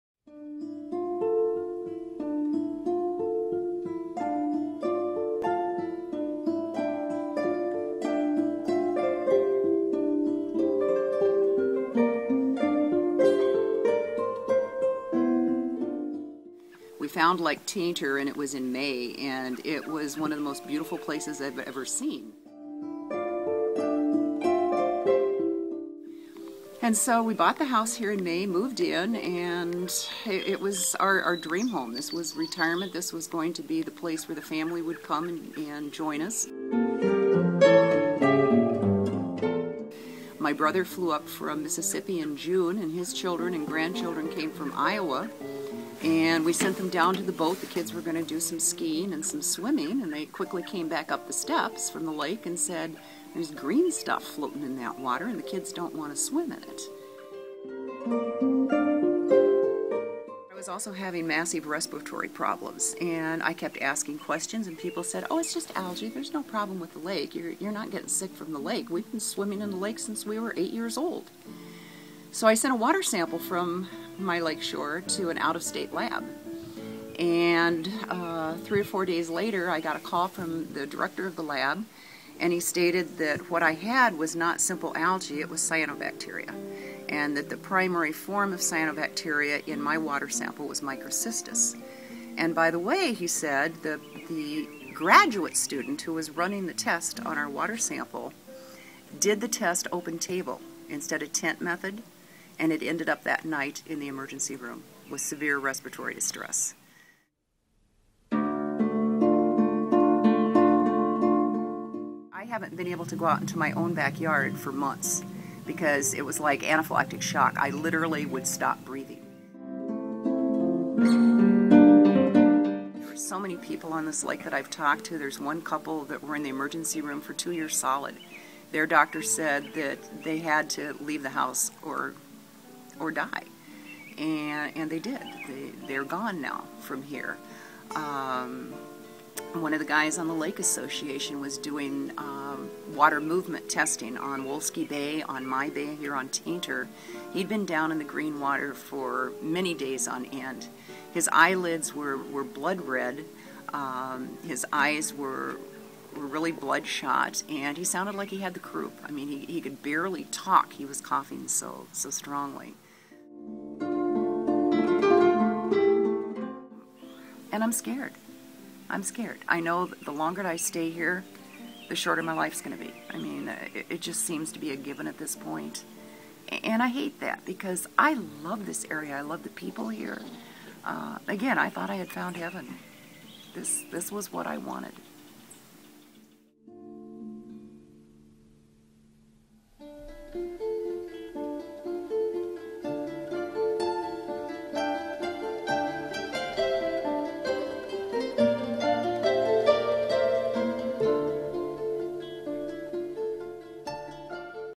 Peggy's Story: Getting sick of phosphorus pollution
Last week Clean Wisconsin visited the lakes of Tainter and Menomin. Every year they're impacted by toxic blue-green algae. We were left in disbelief at the stories that we heard. Now our goal is to share these stories... Today we bring you an interview with Peggy McAloon, a resident of Lake Tainter.